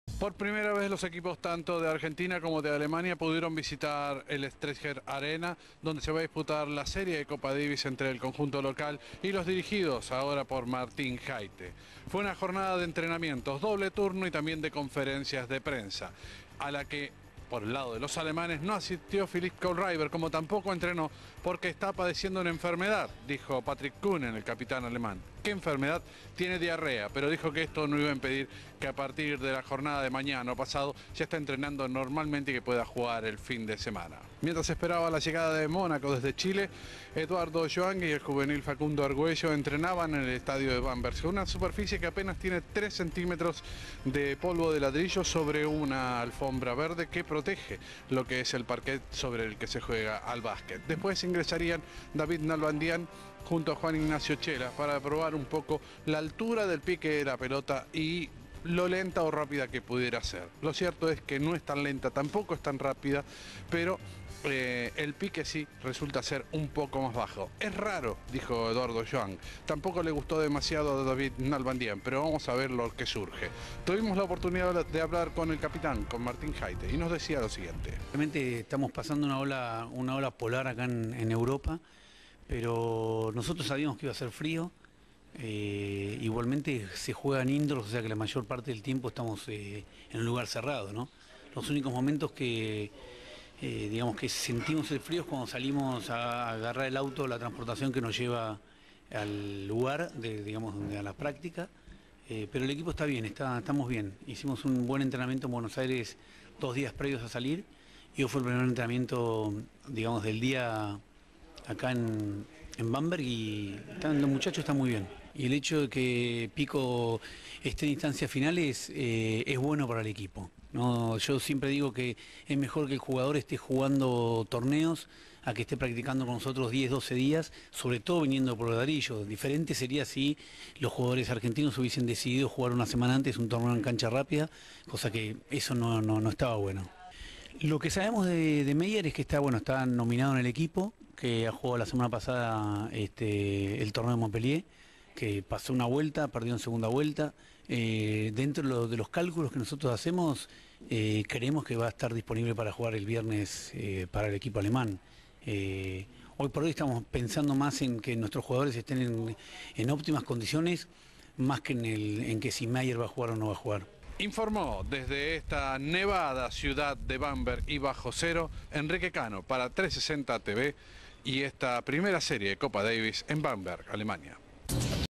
360 TV - Deportes: Martín Jaite en la previa de la Copa Davis
Enrique Cano, enviado especial en Alemania